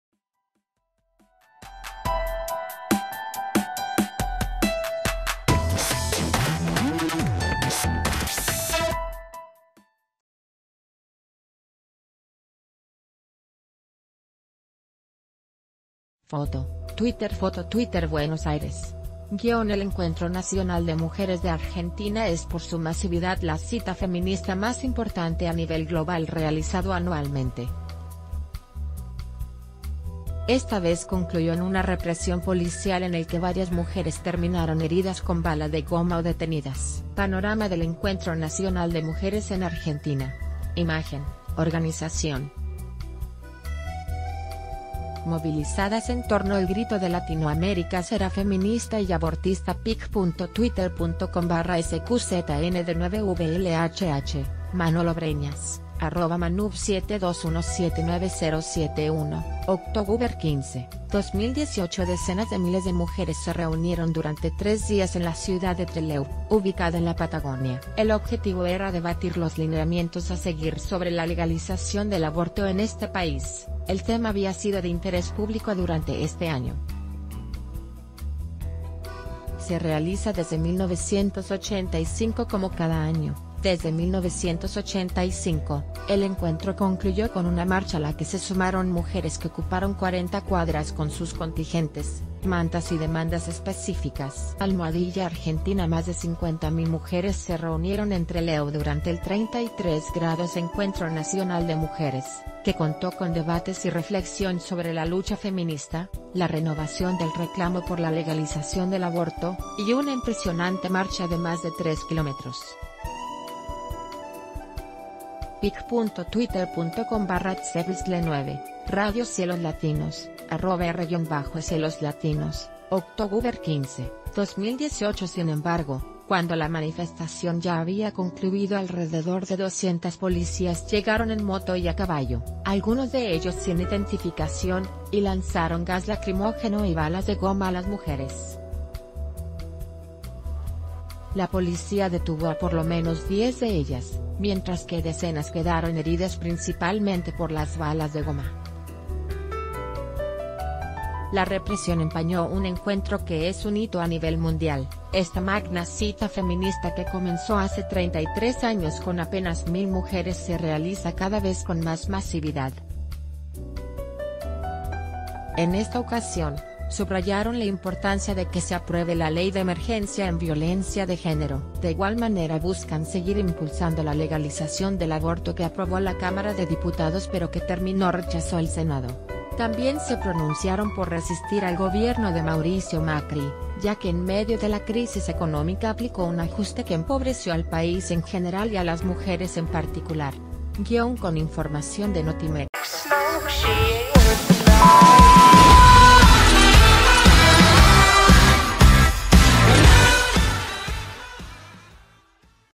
Policía de Argentina reprime histórico encuentro feminista
FOTO: TWITTER FOTO:TWITTER BUENOS AIRES.-  El Encuentro Nacional de Mujeres de Argentina es por su masividad la cita feminista más importante a nivel global realizado anualmente. Esta vez concluyó en una represión policial en el que varias mujeres terminaron heridas con bala de goma o detenidas. Pan...